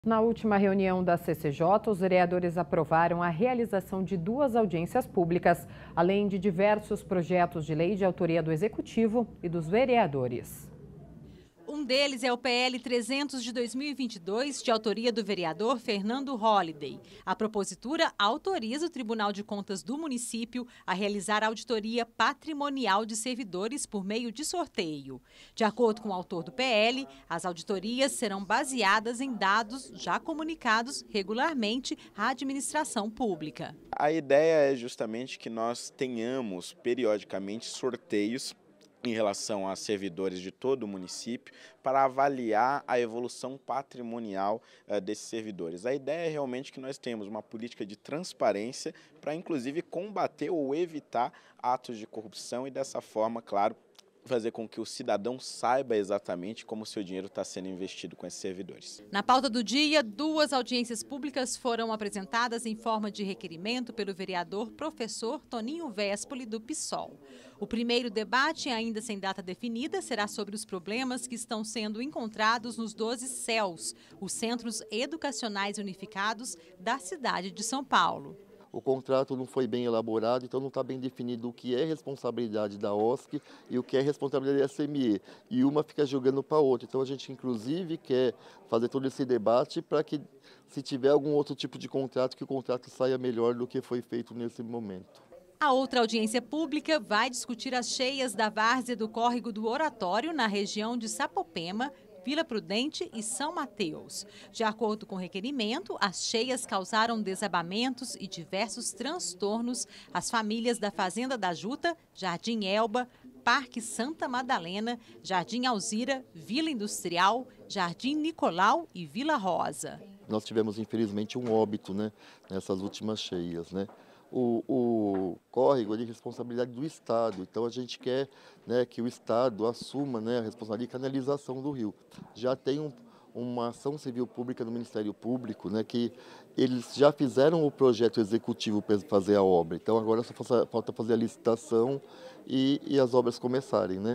CCJ aprova PL de auditoria surpresa e mais duas Audiências Públicas
Na última reunião da CCJ, os vereadores aprovaram a realização de duas Audiências Públicas pelo colegiado e diversos Projetos de lei de autoria do Executivo e dos vereadores. Um deles é o PL 300/2022, a propositura autoriza o Tribunal de Contas do Município a realizar auditoria patrimonial de servidores por meio de sorteio. De acordo com o autor do PL, as auditorias serão baseadas em dados já comunicados regularmente à administração pública.